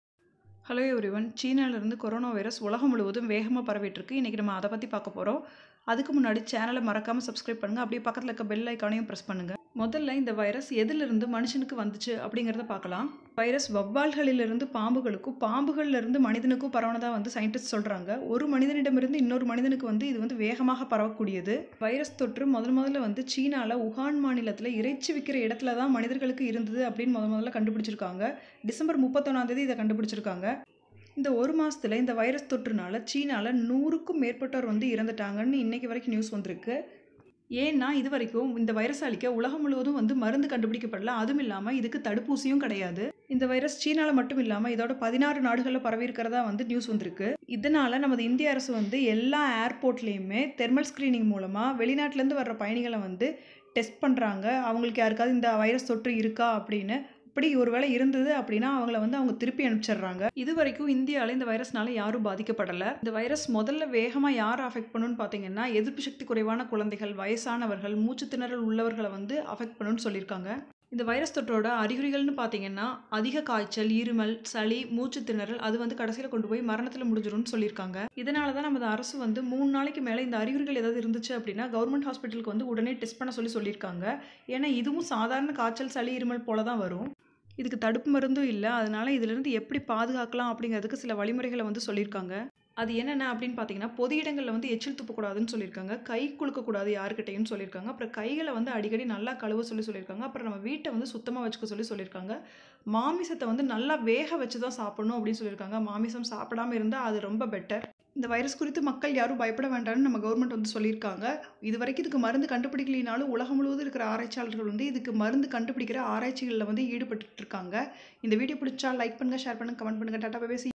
Corona virus in tamil-கொரானா வைரஸ் பயம் தமிழில்
This video contains details about corona virus from starting to still now